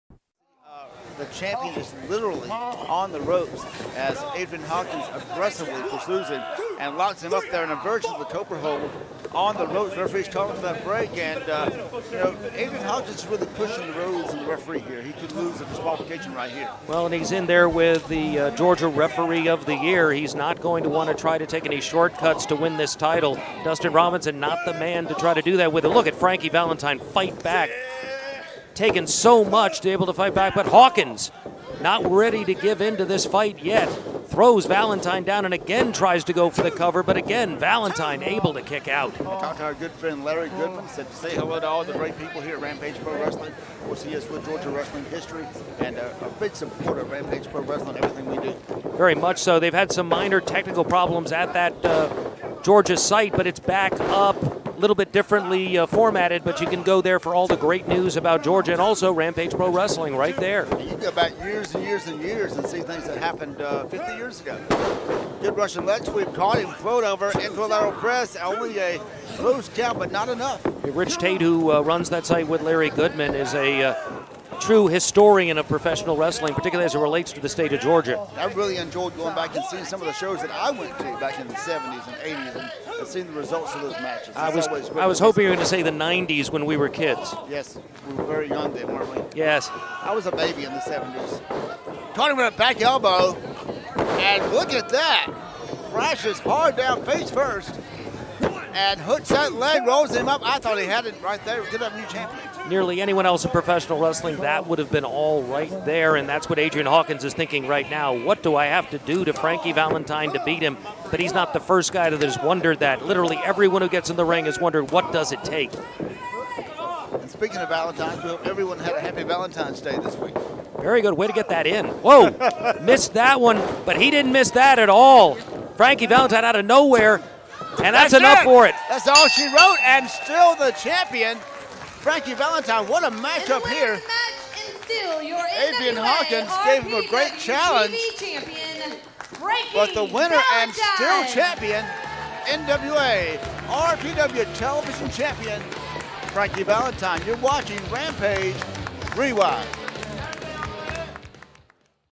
Frankie Valentine (c) vs Adrian Hawkins TV Title Match NWA RPW 89 2-12-12
TV Title Match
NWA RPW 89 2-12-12
Ref: Dustin Robinson
Announcers: Bill Behrens, Ben Masters & Miss Allie